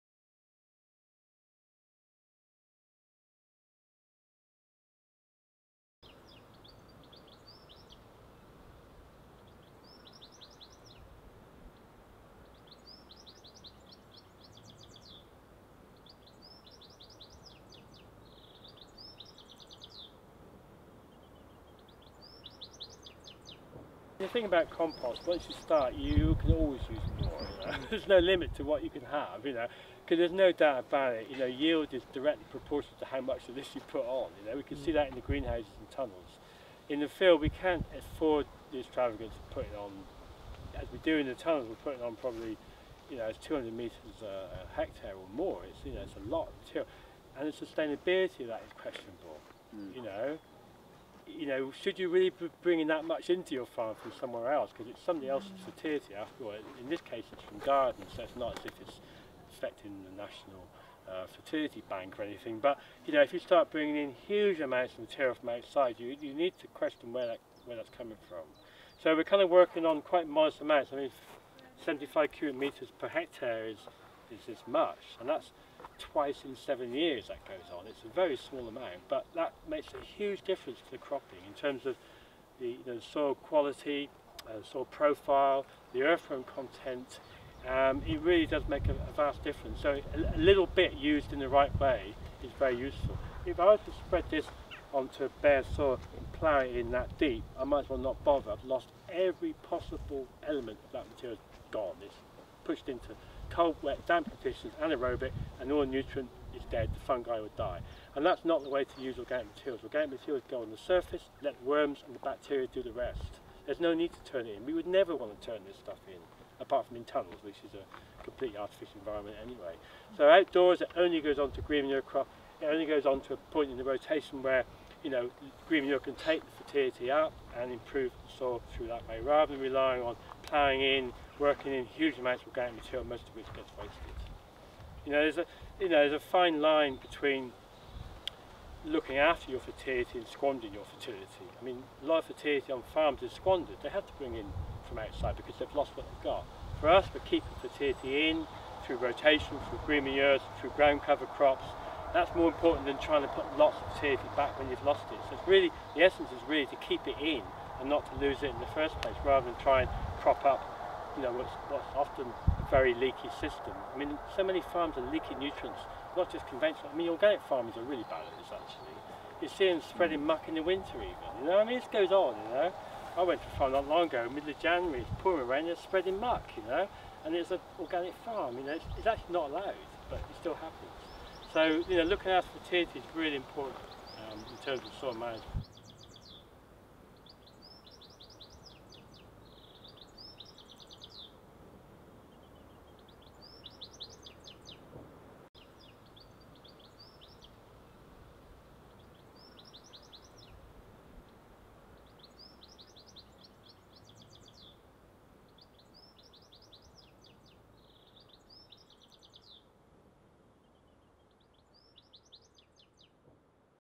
Using wood chips in soil fertility management Tolhurst Organics, UK
Iain Tolhurst explains the important role of woodchip compost  in maintaining soil fertility on his organic vegetable farm in Whitchurch-on-Thames, UK